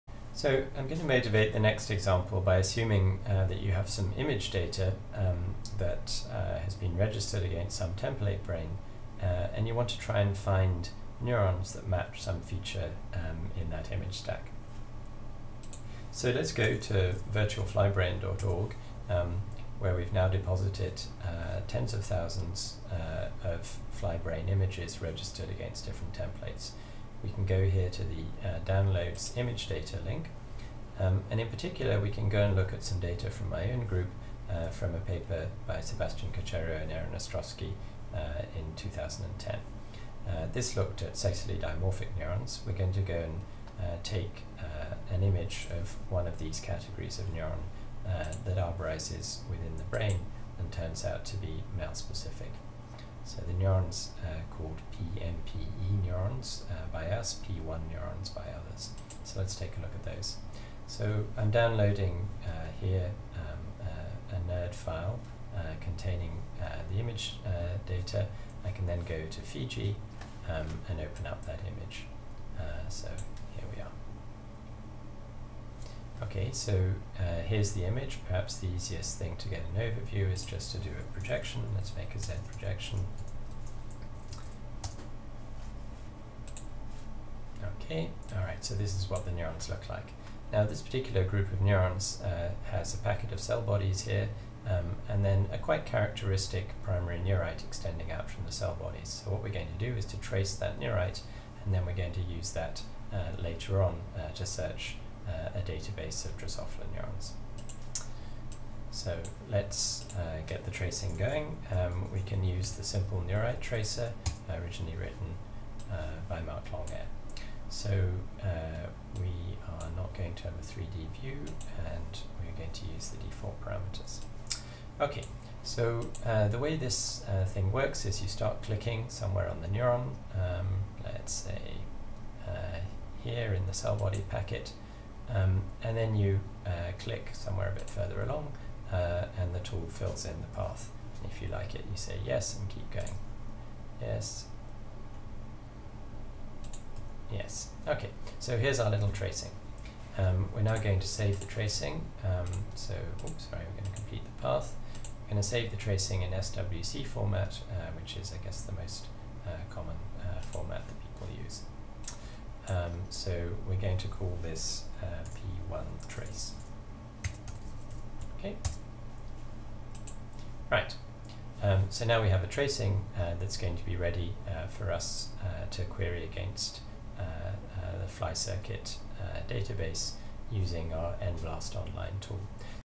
Making a tracing from registered image data
and show how to make a 3D tracing of a feature in that image. This can then be used as a search input for NBLAST.